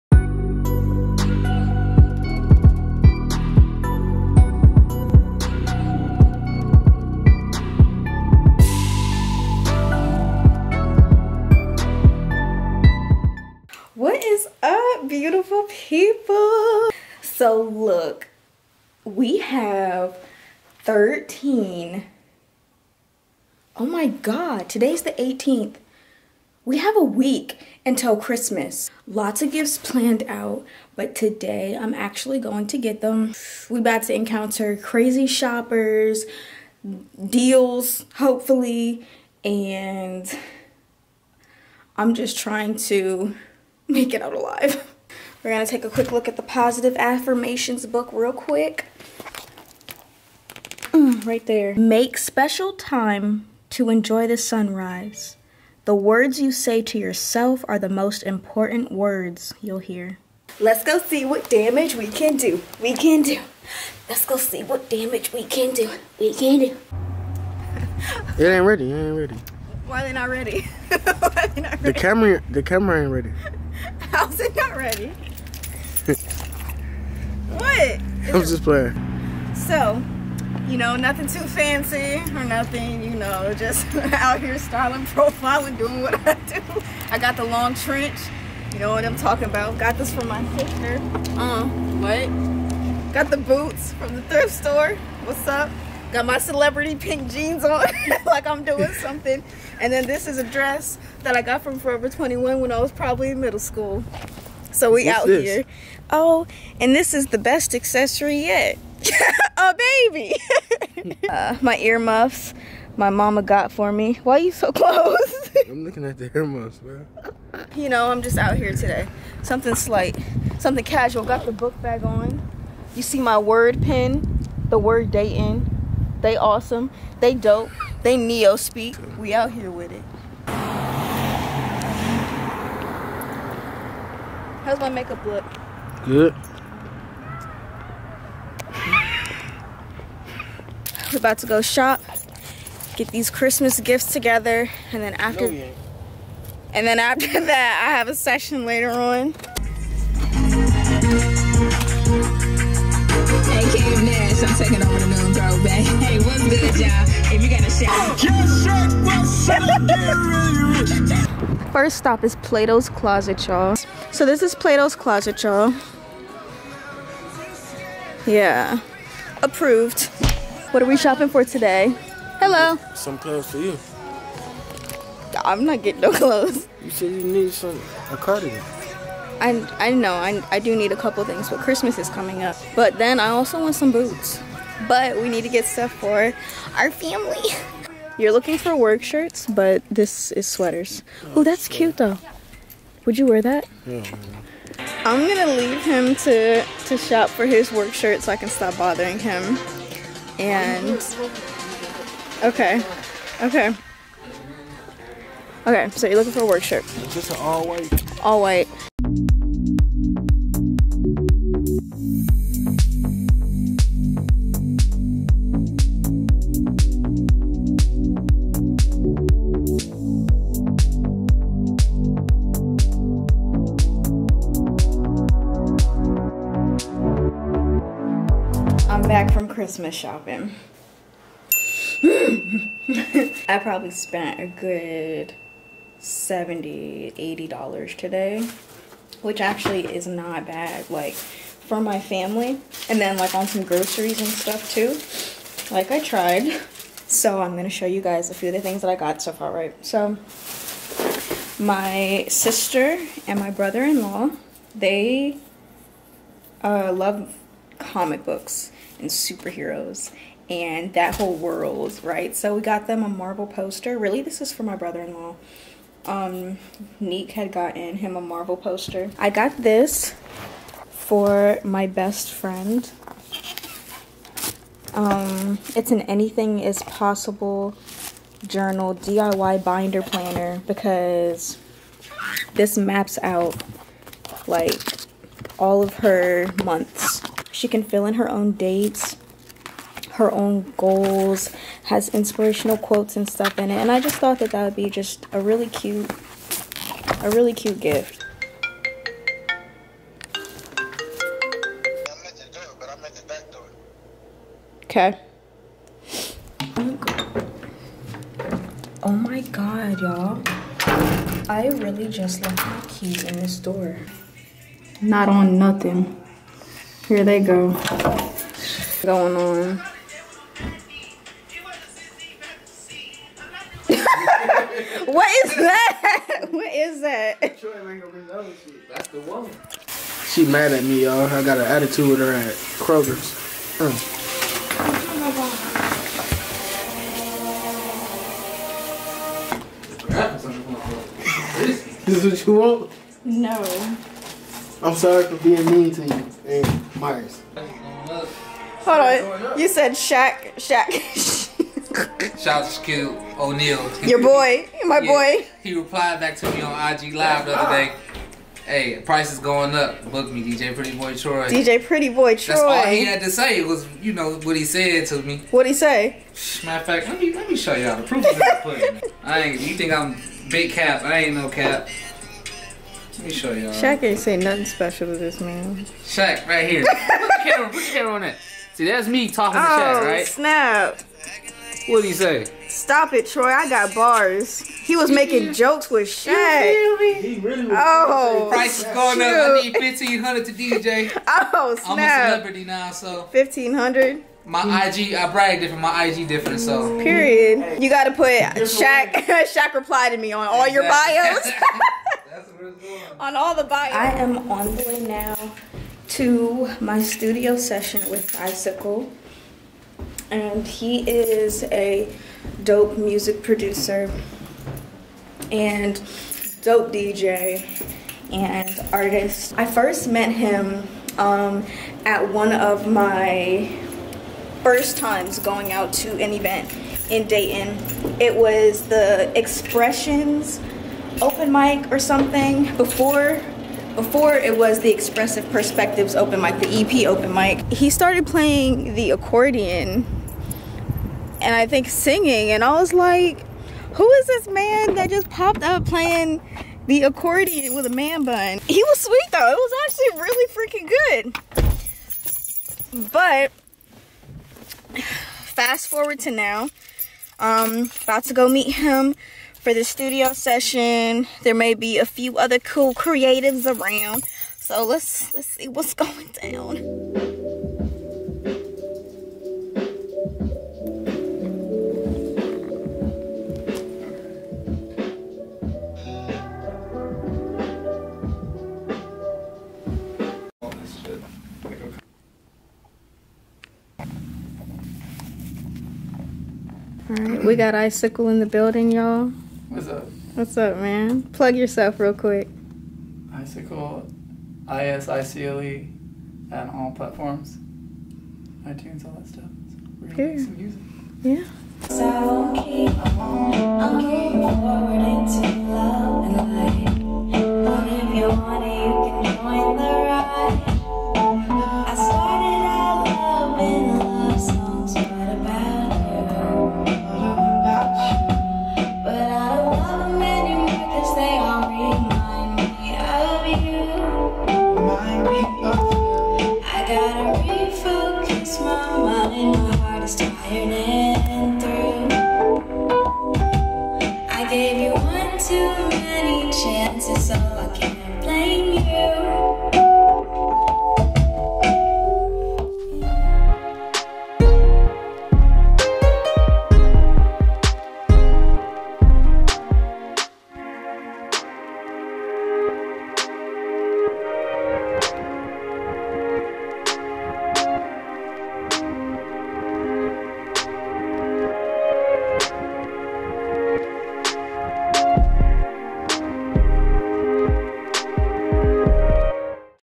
(Free Episode!!!) Marah's Way Day by Day | Season 2 Ep. 4
** EXPAND FOR ADDITIONAL INFO**

Hey Guys! I decided to give you guys a taste of what's in Season 2 of my vlog series on Xperienc on Demand! Just for you ;)

Marah and Dom go Christmas shopping, DJPrettyBoyTroy makes an appearance, and Marah records a new banger with Isicle.

Save $$$ on your RockMyBirth.com purchase with code ' BABYPOE '
Save 10% on your ShopChocoGirls.com order with code ' XOSPOE '

if you're reading this ilyyy